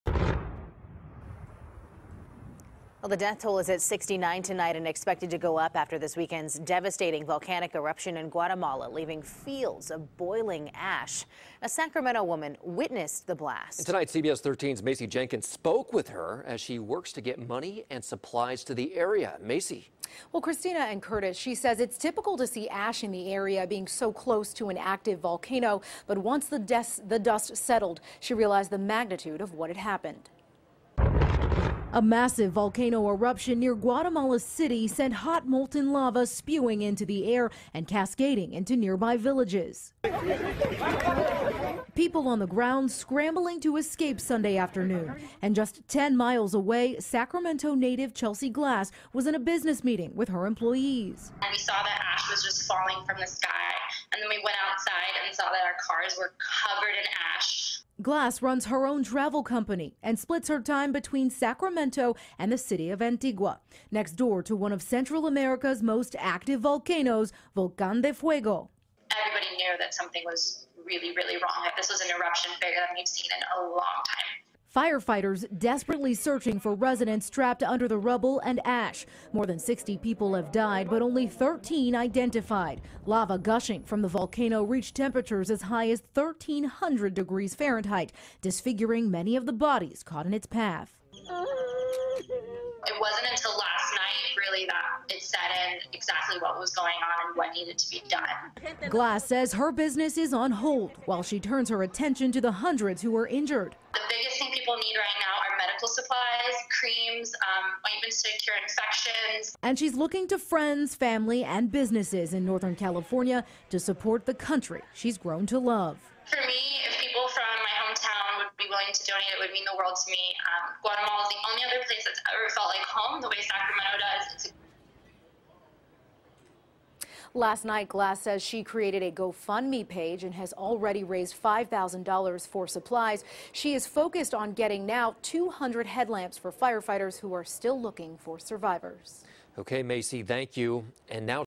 Sacramento Woman A Witness To Deadly Guatemala Volcano Eruption
Chelsea Glass was in a business meeting with her employees just 10 miles away from the blast. Ash was falling from the sky.